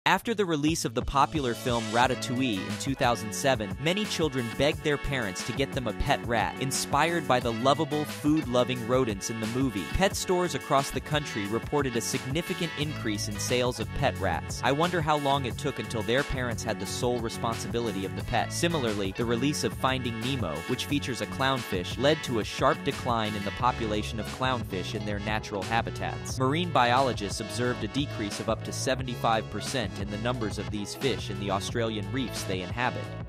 incedible Ratatouille and Finding Dory Fun Fact
Did you know that the release of popular movies can have a significant impact on pet and wildlife populations? In this video, we explore two examples: the surge in demand for pet rats after the release of Ratatouille, and the sharp decline in clownfish populations following the popularity of Finding Nemo. We delve into the fascinating reasons behind these phenomena and discuss the ethical implications of our cultural fascination with certain animals. Join us for a thought-provoking discussion on the influence of media on our relationship with animals.